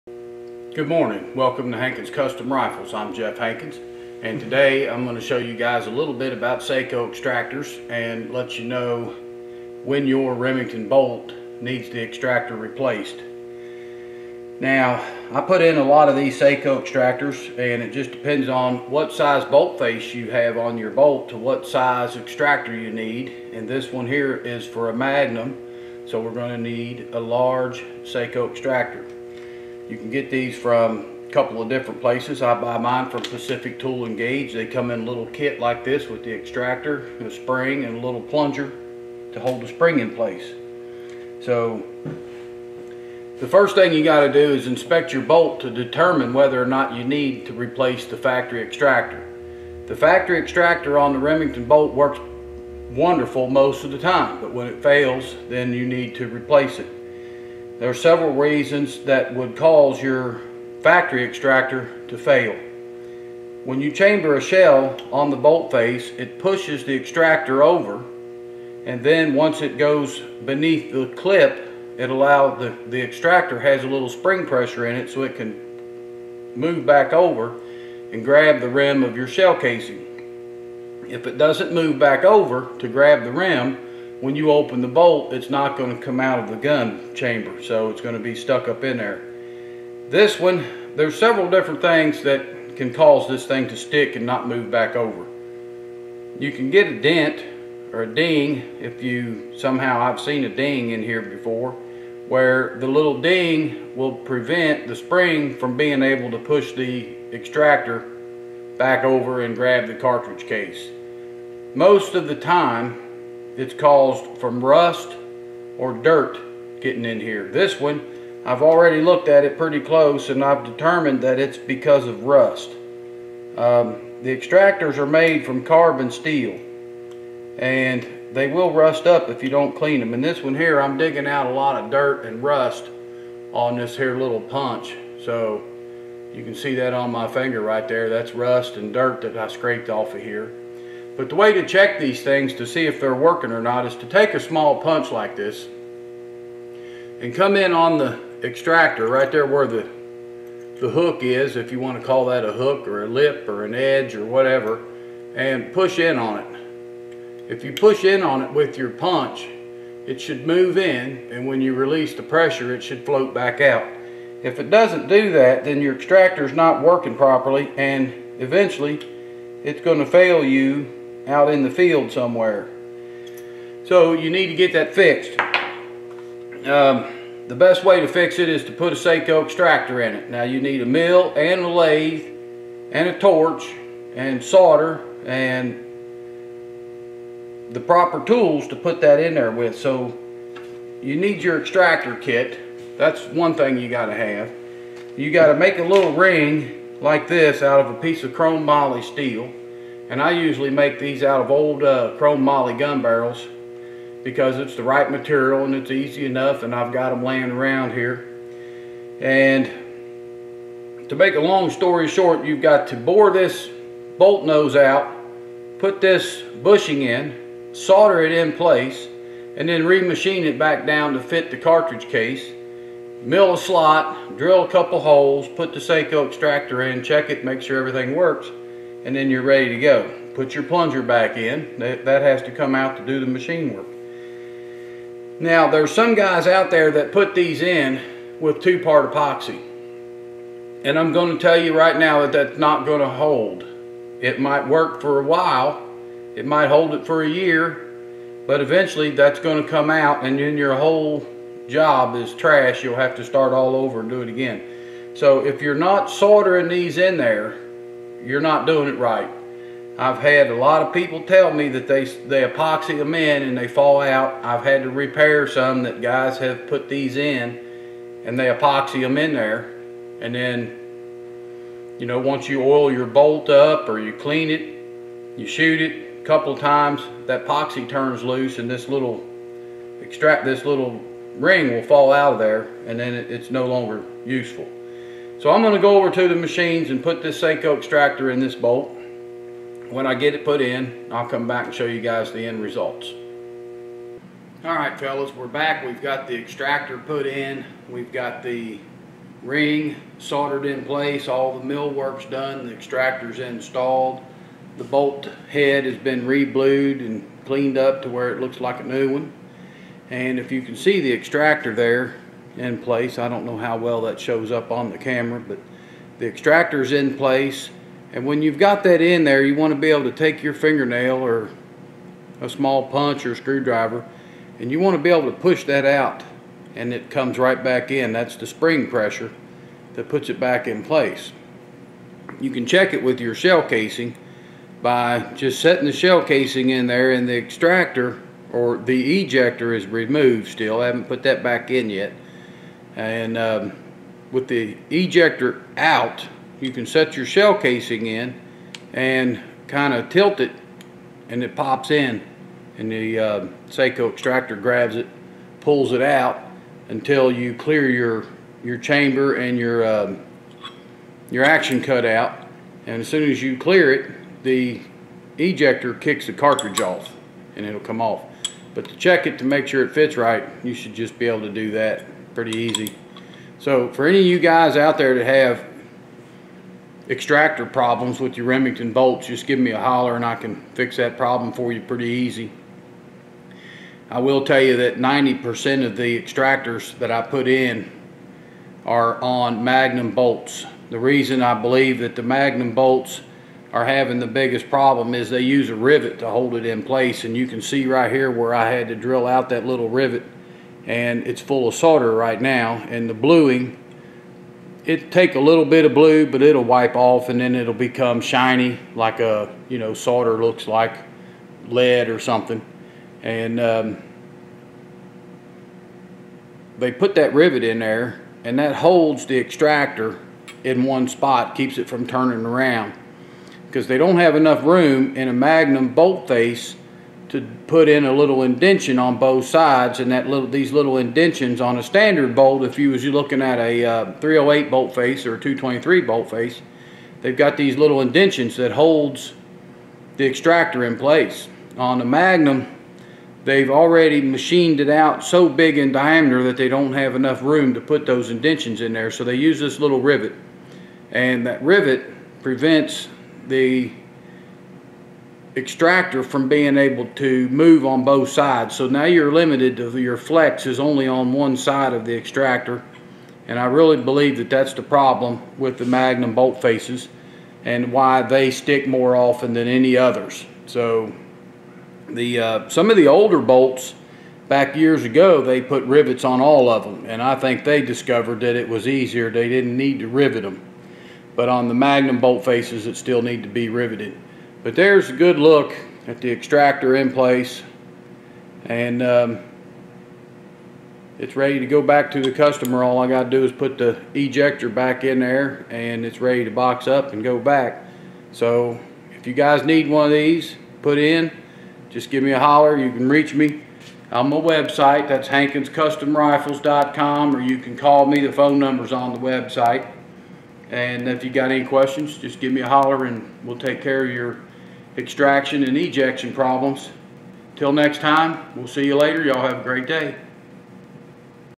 Sako Extractor
In this video I tell you a little about Sako extractors and how they work, how to identify a  defective factory extractor and when you should consider replacing it.